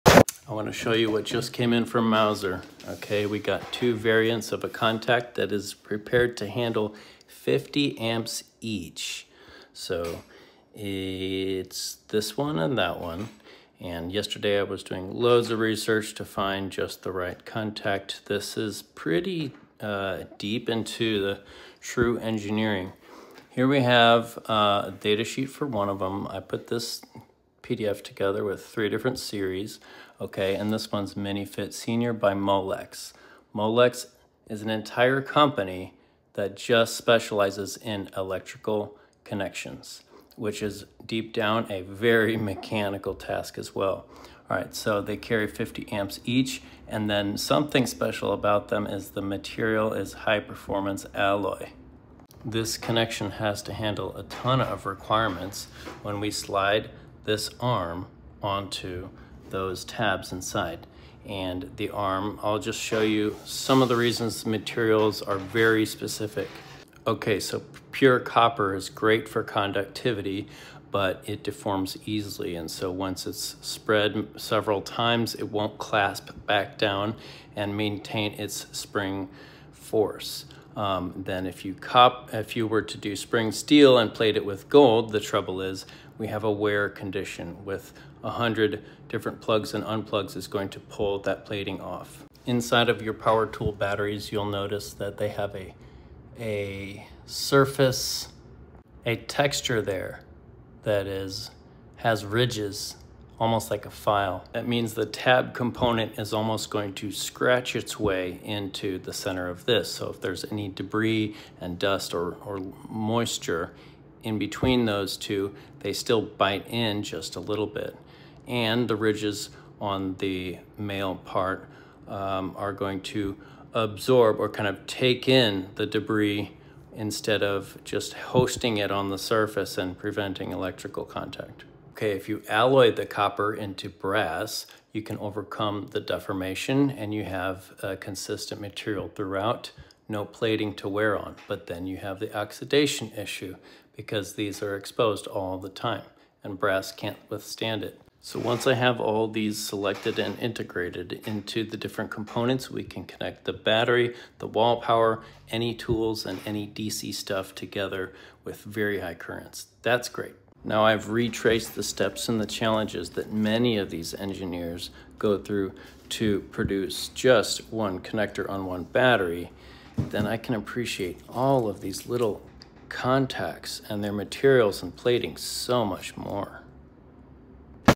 complex engineering of a high-current battery contact, for power tools
This activity comes from our efforts in the OpenLab Project as well as SCUTTLE Robotics.  Modern batteries are modular, and this particular Ridgid battery design is suitable for thousands of mechatronics engineering designs if we can produce our own custom adapters to fit together the power tool battery and a receiving-side connector.

The video features the tab of a ridgid li-ion 4AH battery which suits many power tools.  The tab is common among most power tool batteries and unlike AC appliances, the intended amperage maximum is MUCH HIGHER at 50 Amps per terminal.

I'm compiling documentation about the battery terminal selection for DIY makers and engineers to select their own terminals, and including the best choices of OTS tabs  by manufacturers Molex and TE connectivity.  In April 2025 I collected datasheets that I'll post on my grabCAD design as well as in OpenLabProject.   Ultimately we want Open Source adapters that suit any DC project to be made Cordless, and to make any battery brand swappable with any power tool brand.